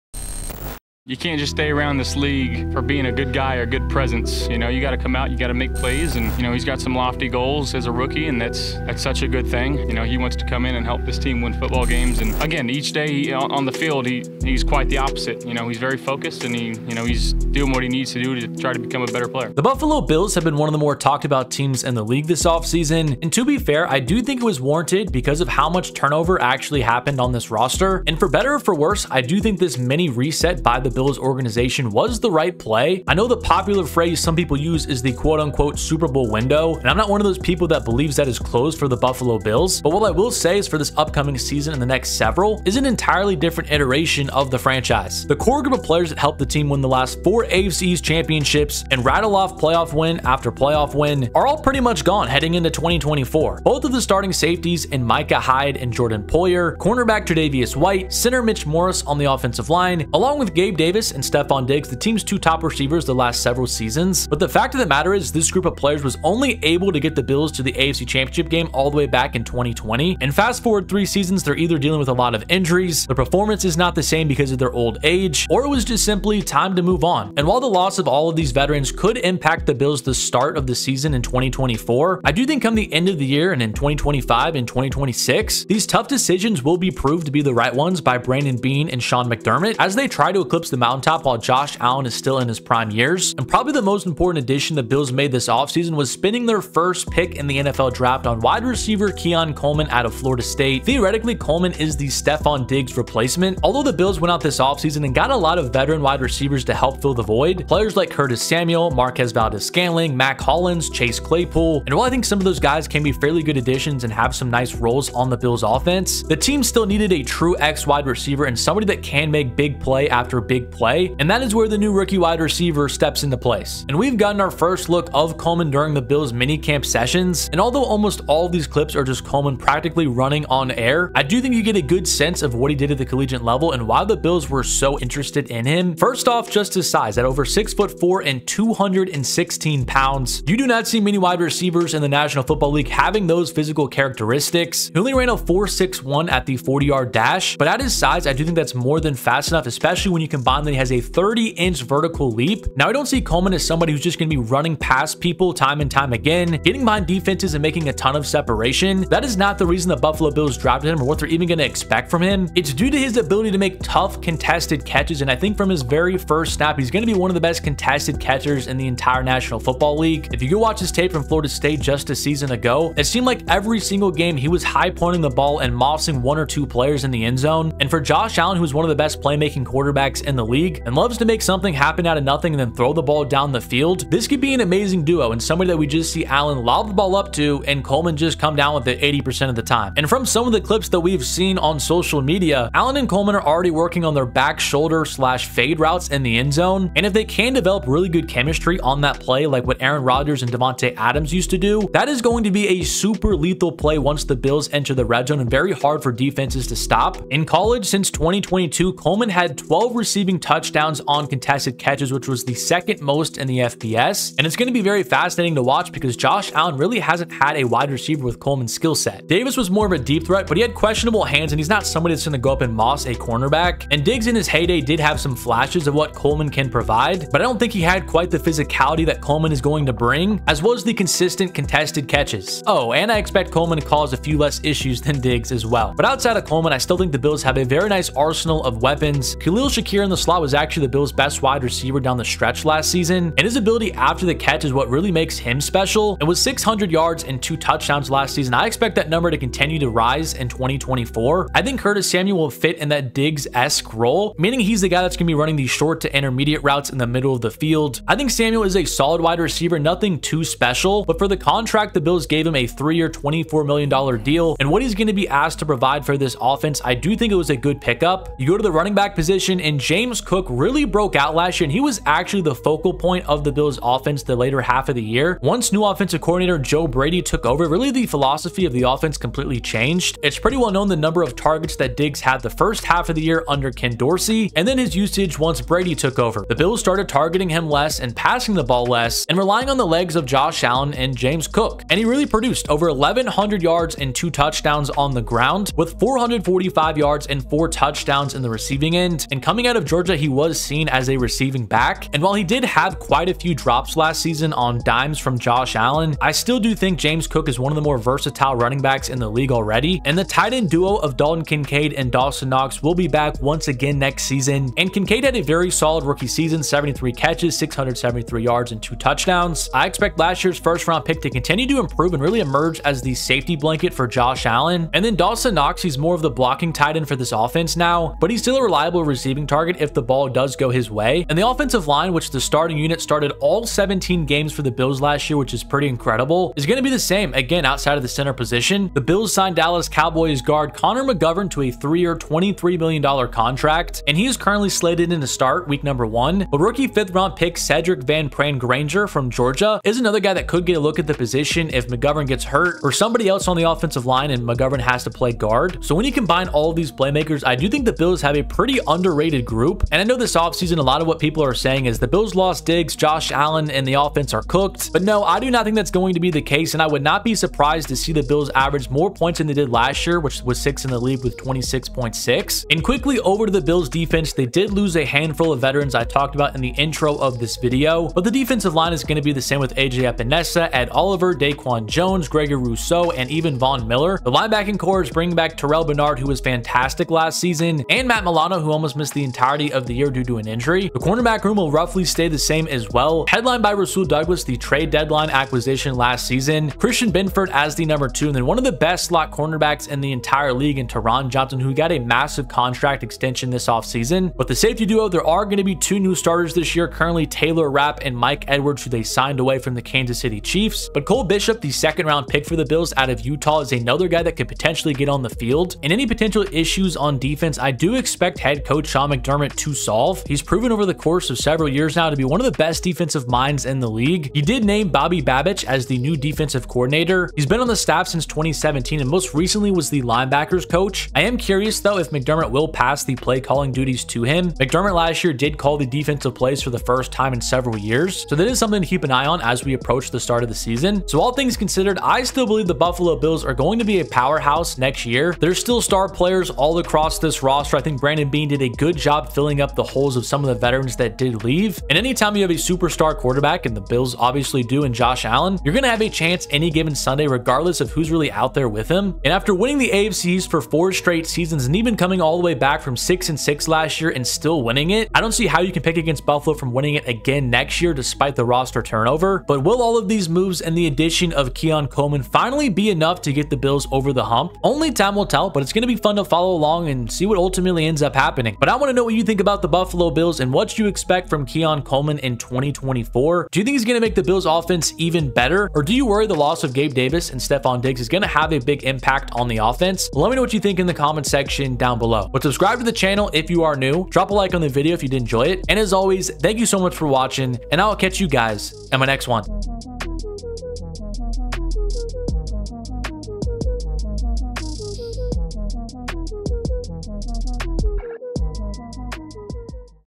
Keon Coleman Is EXACTLY What The Buffalo Bills Needed
I hope you enjoyed today's video about Keon Coleman & the Buffalo Bills.